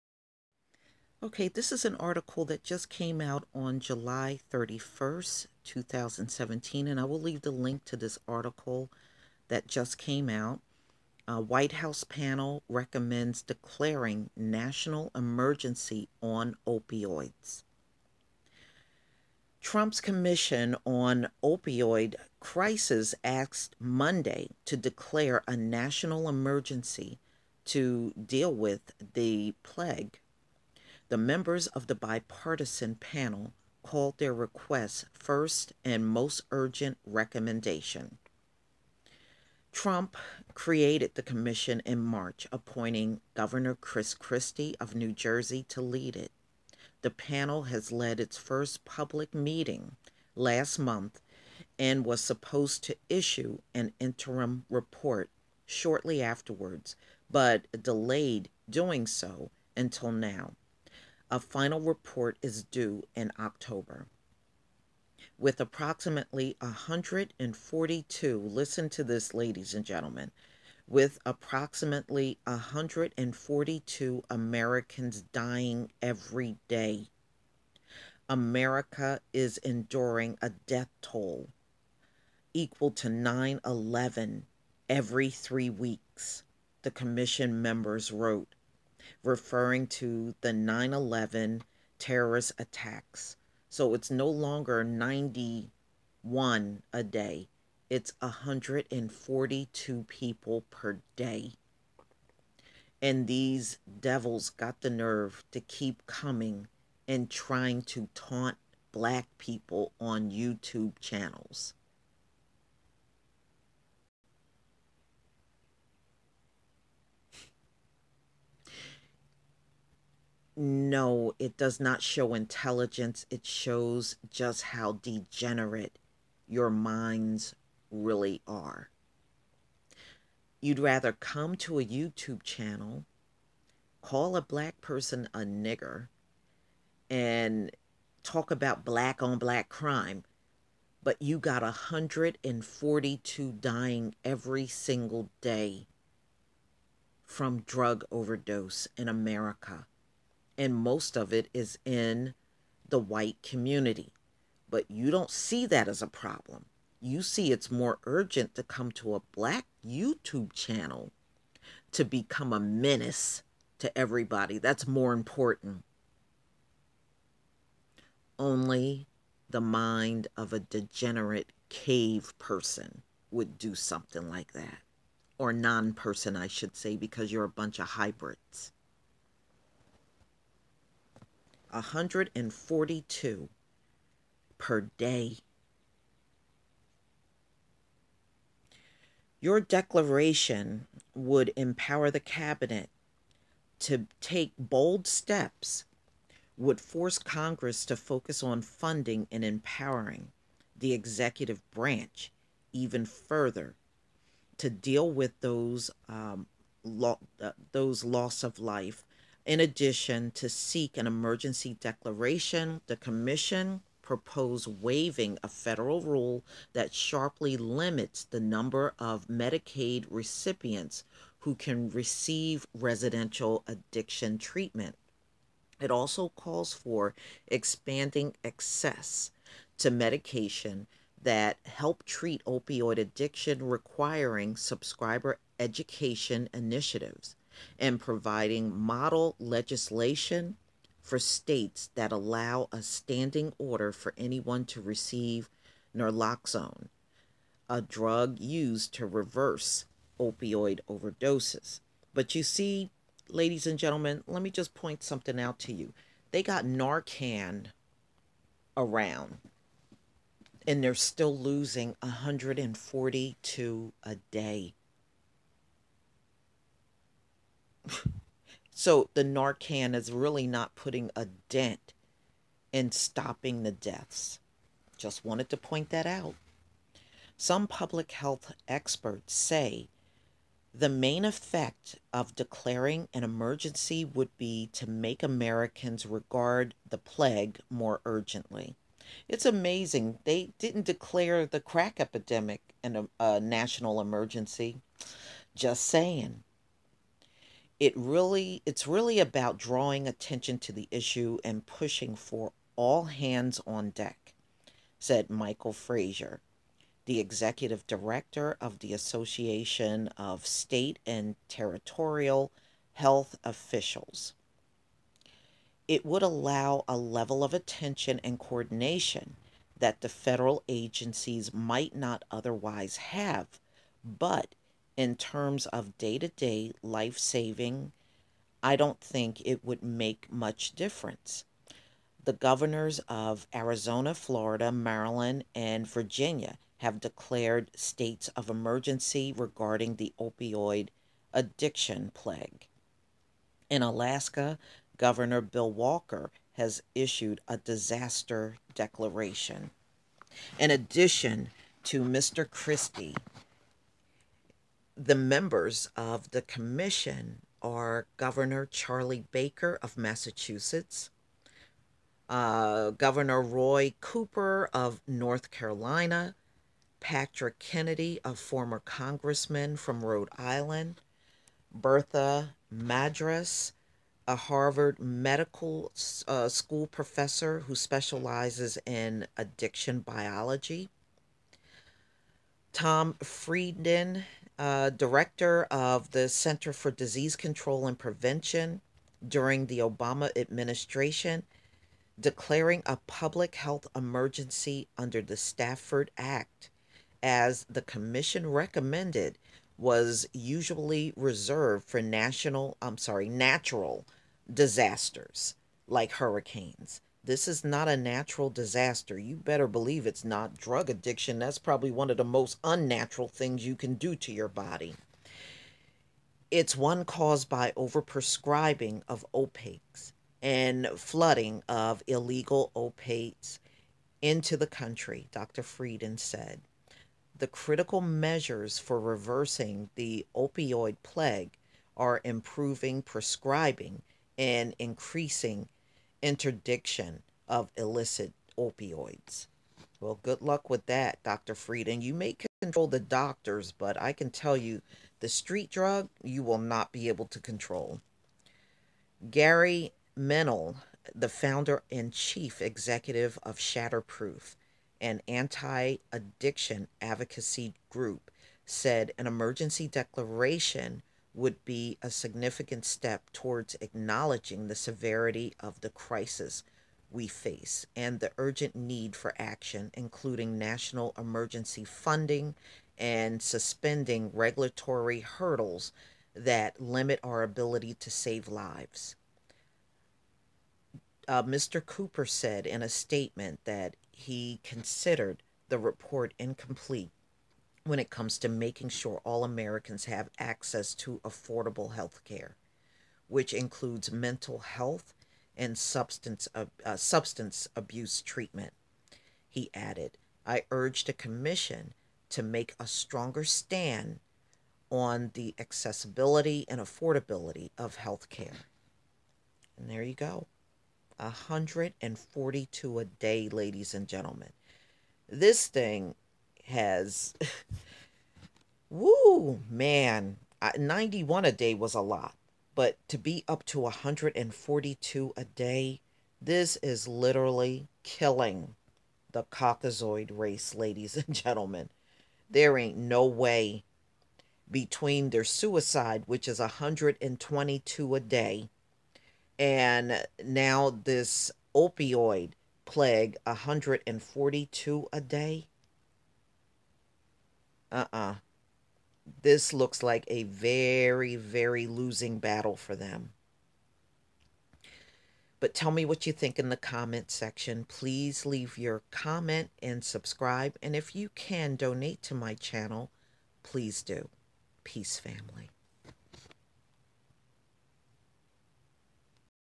Opioid Deaths Up To 142 Per Day In America
White House Panel Recommends Declaring National Emergency on Opioids